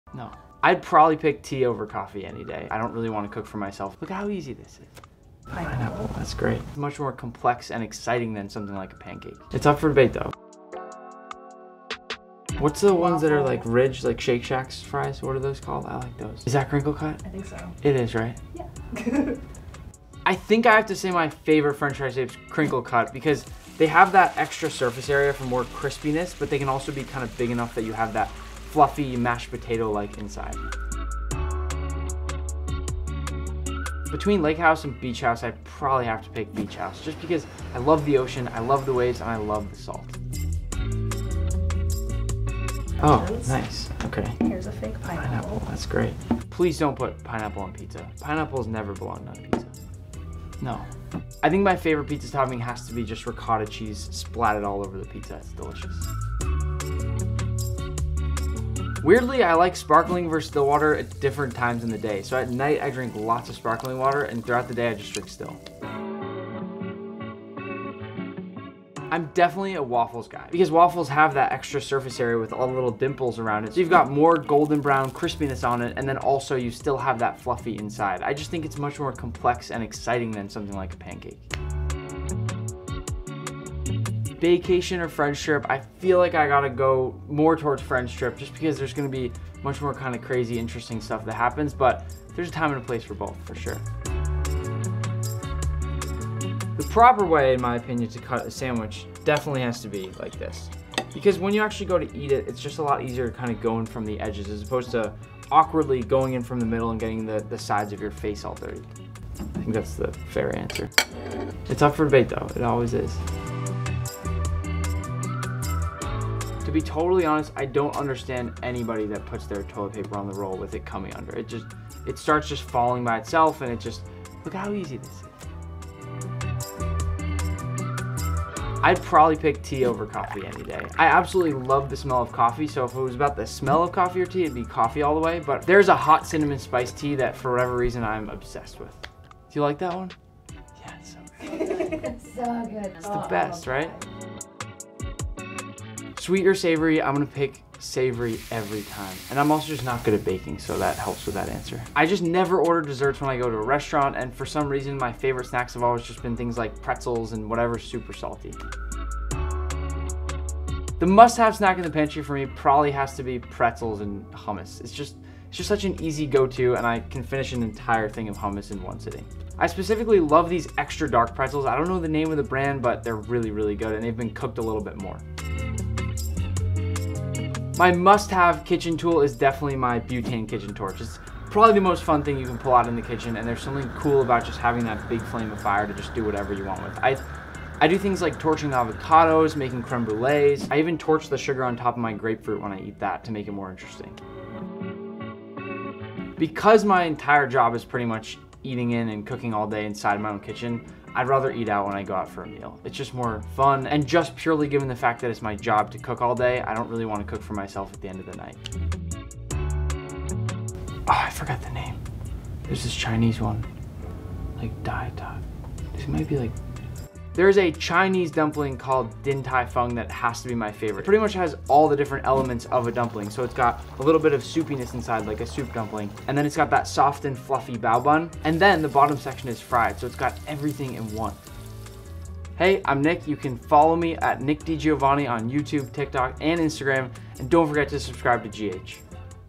Why Nick DiGiovanni Loves To Blow Torch His Breakfast | Good Housekeeping
Chef Nick DiGiovanni tells us all about his favorite dumplings, the ideal french fry shape and even why he loves using his blow torch for all his meals.

Follow Nick on YouTube @NickDiGiovanni.